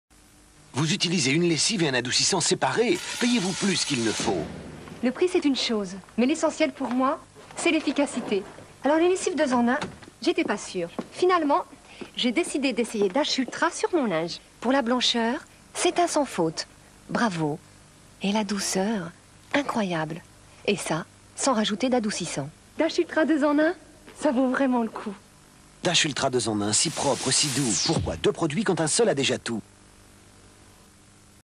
Publicité 1992 Dash Ultra 2 en 1 (Case Cochée)
Publicité de 1992 pour la lessive Dash Ultra 2 en 1.
Bon Visionnage.

Et n'oubliez pas, 5 nouveaux spots chaque jour à 00h00.
7 Jours sur 7 / 365 jours par an.
Merci.